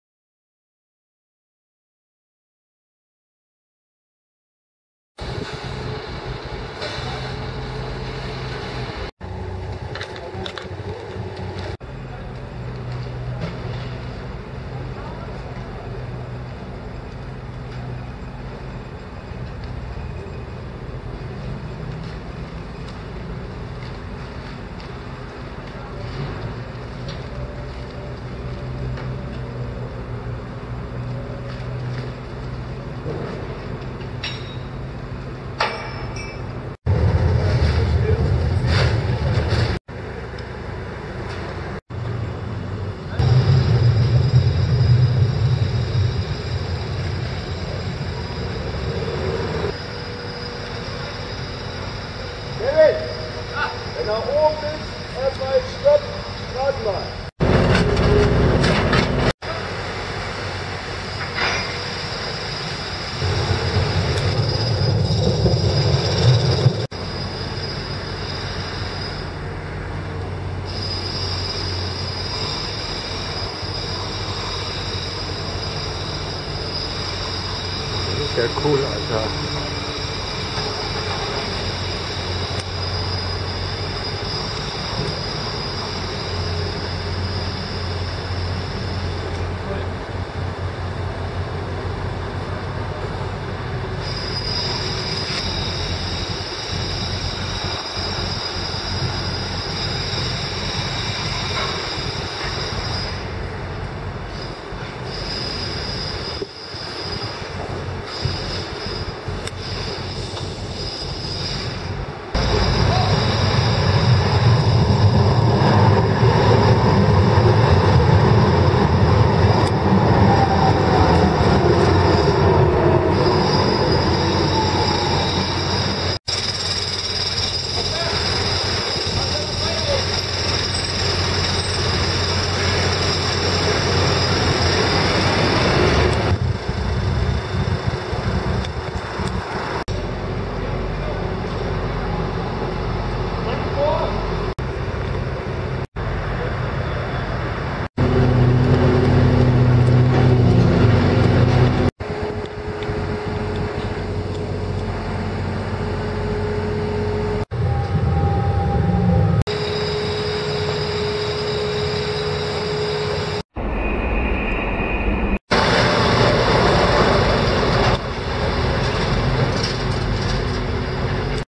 Liebherr LTM Rolltreppe Wehrhahn Linie Oberleitung
Mit Baufortschritt werden die Rolltreppen in die U-Bahn Linie eingebaut. Düsseldorf